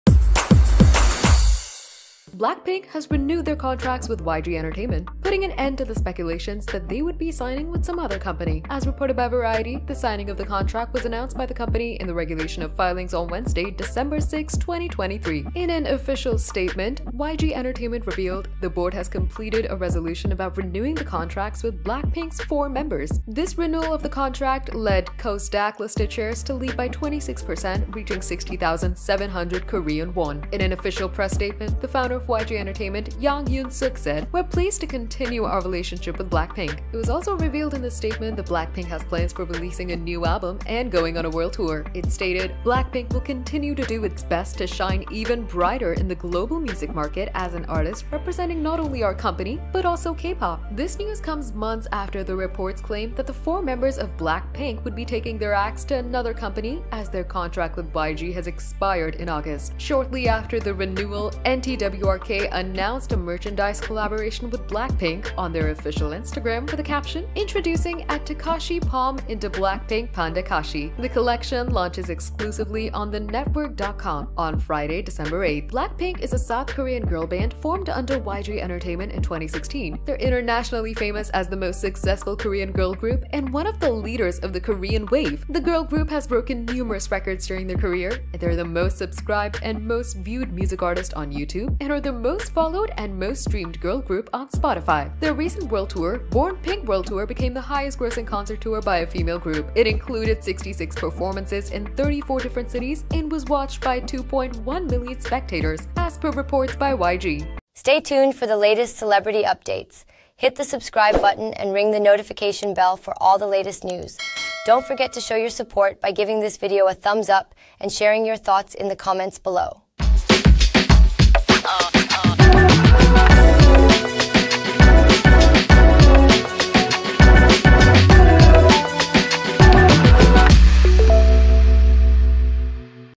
BLACKPINK's Epic Comeback Sparks Excitement | Gossip Herald
BLACKPINK renewed contract with YG Entertainment on December 6 after its expiration in August 2023.